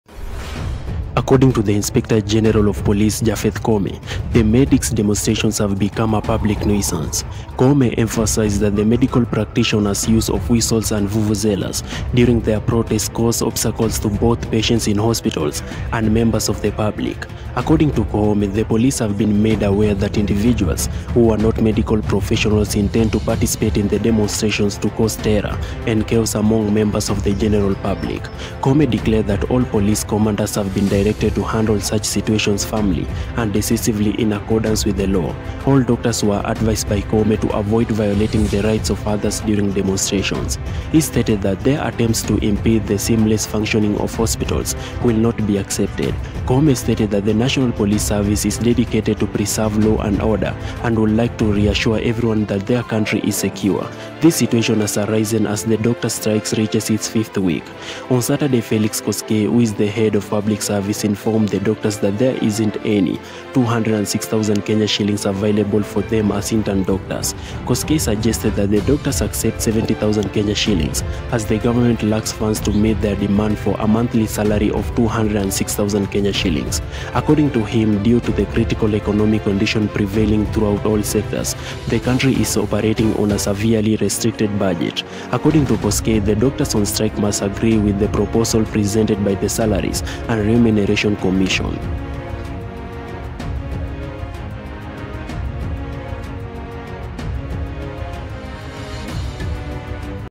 IG Koome warn doctors on strike
according to the Inspector General of Police Japheth Koome, the Medics' demonstrations have become a public nuisance.
Koome emphasized that the medical practitioners', use of whistles and vuvuzelas during their protests, causes obstacle to both patients in hospitals and members of the public.